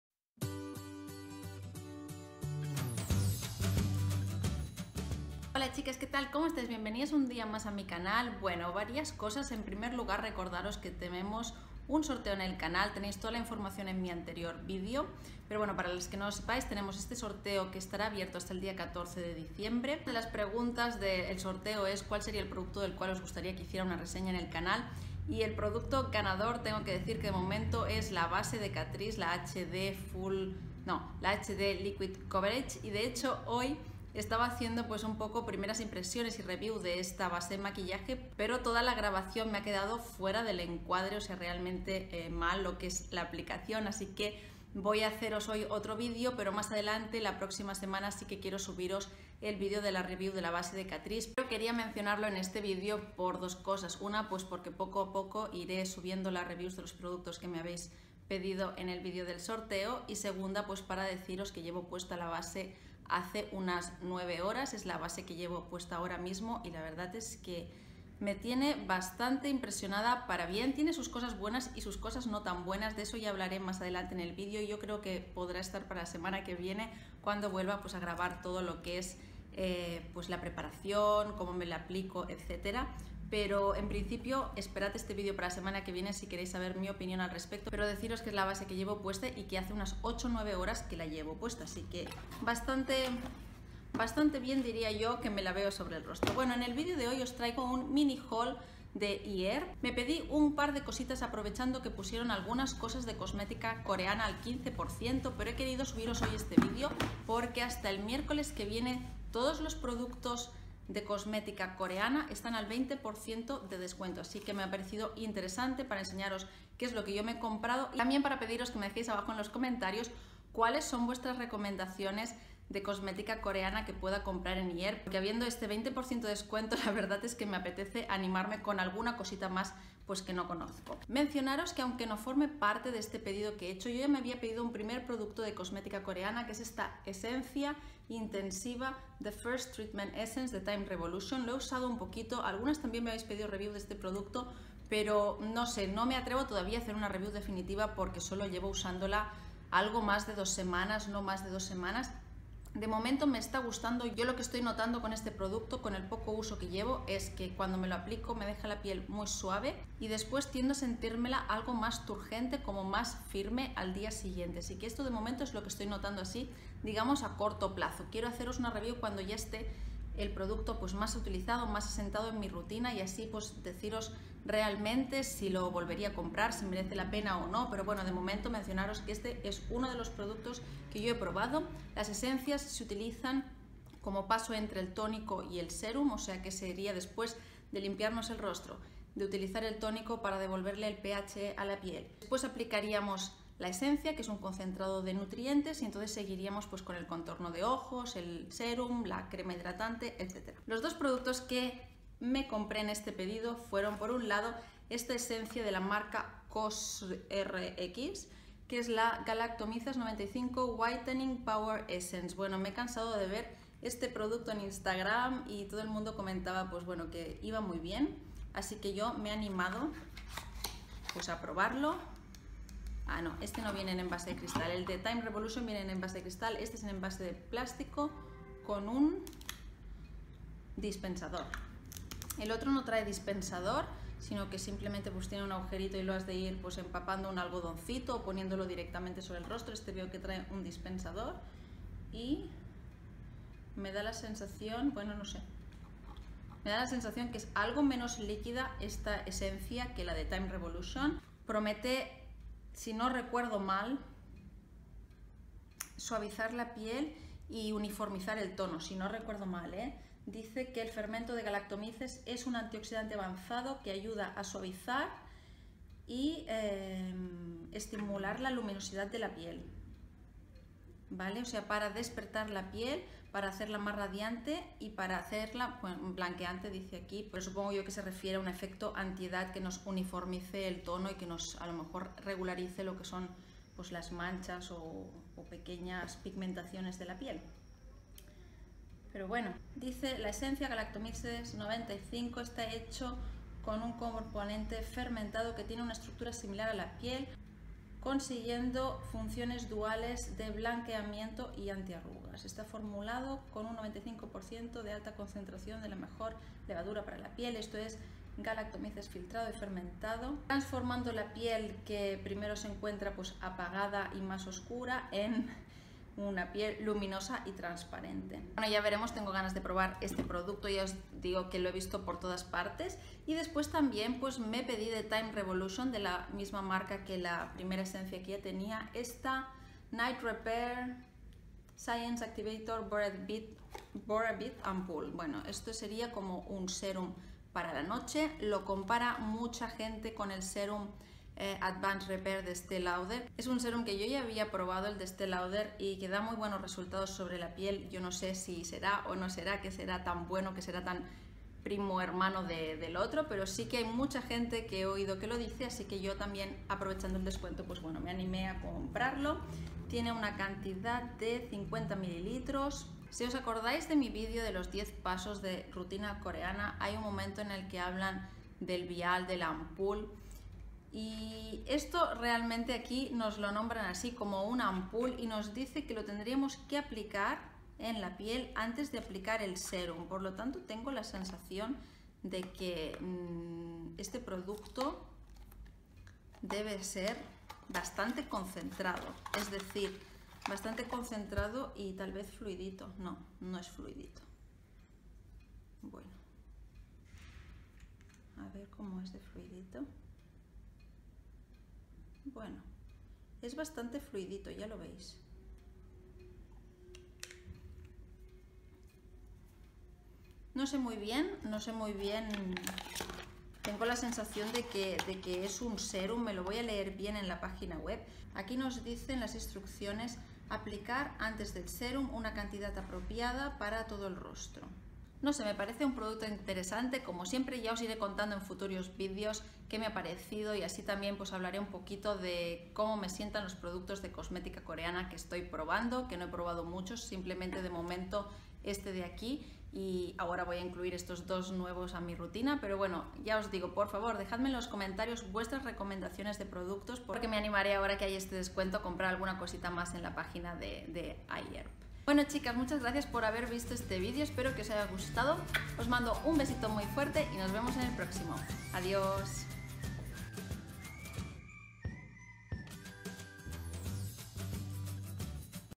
Haul Iherb: Cosmética Coreana
Hola hermosas,

Os traigo un breve vídeo con mis compras de cosmética coreana a la página Iherb. Por favor, dejadme abajo vuestras recomendaciones.

💄💄💄Sobre los ojos llevo Nillionaire de Colourpop y en los labios Calypso, de la misma marca💄💄💄

Os deseo un feliz fin de semana, de muchos mimos y descanso!💆